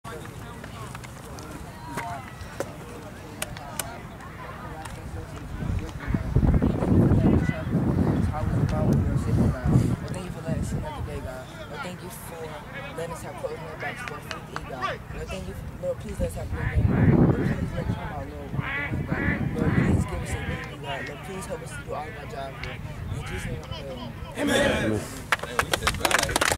Guard 13-14 Pregame Prayer
Corey leads the team in prayer